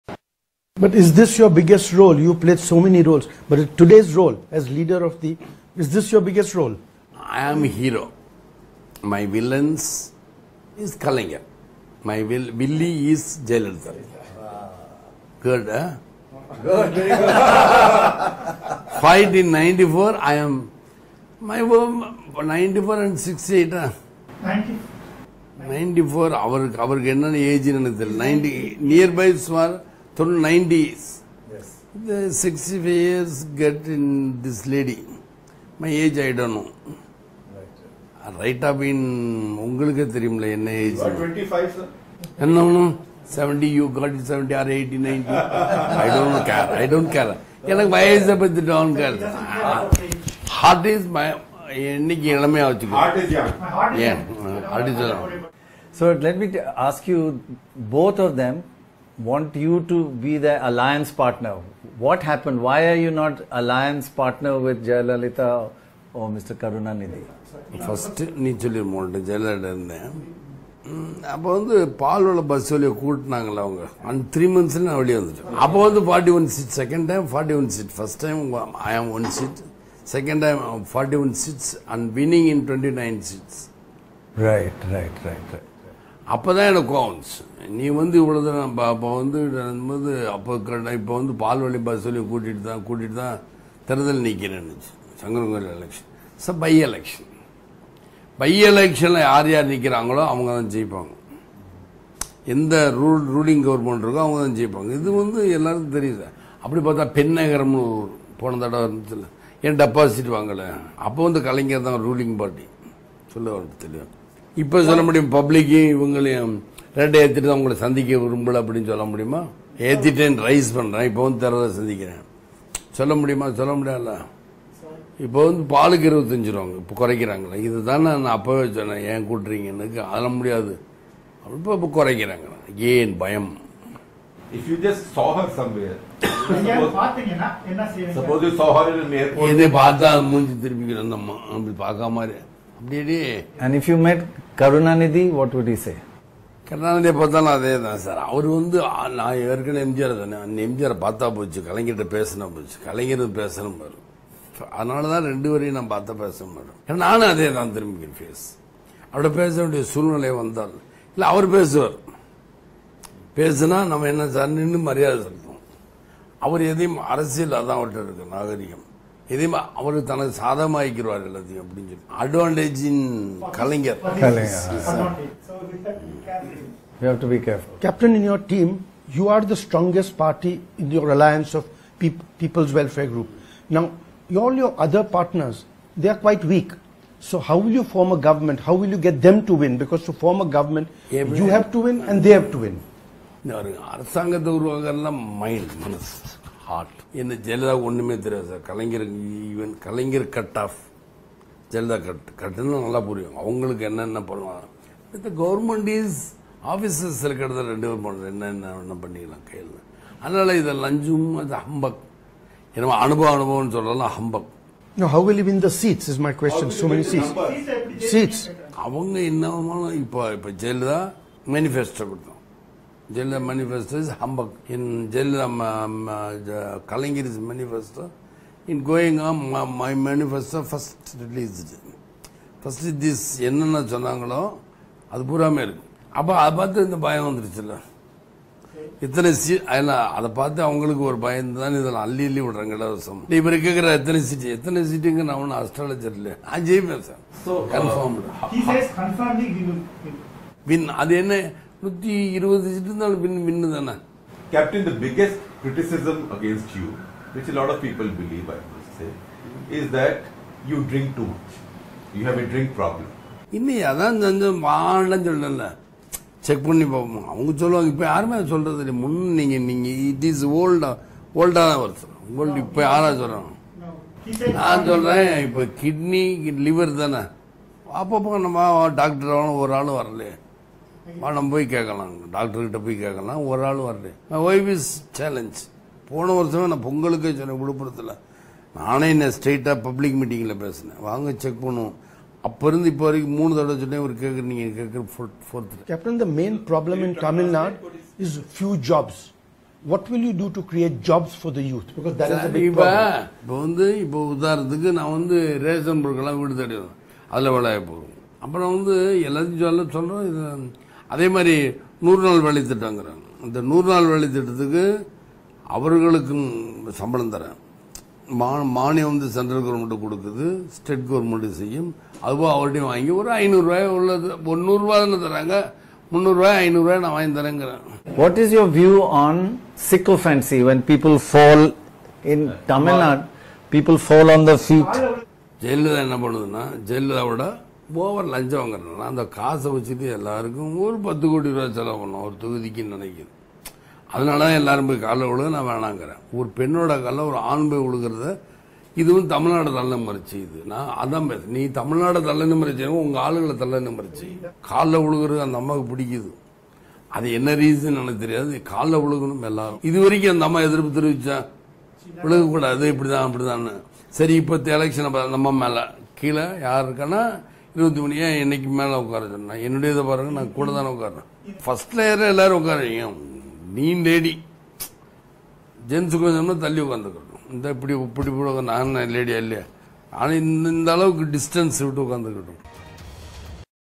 No drinking problem, says 'Captain' Vijayakanth to Prannoy Roy
If politics were a movie, DMK Chief M Karunanidhi would be the villain and Tamil Nadu Chief Minister Jayalalithaa would be a "villie", says actor-turned politician Vijayakanth. He gives no prizes for guessing who the hero is, but reveals why he parted ways with Jayalalithaa, his challenge to those who say he has a drinking problem and his campaign for the 2016 Tamil Nadu assembly elections.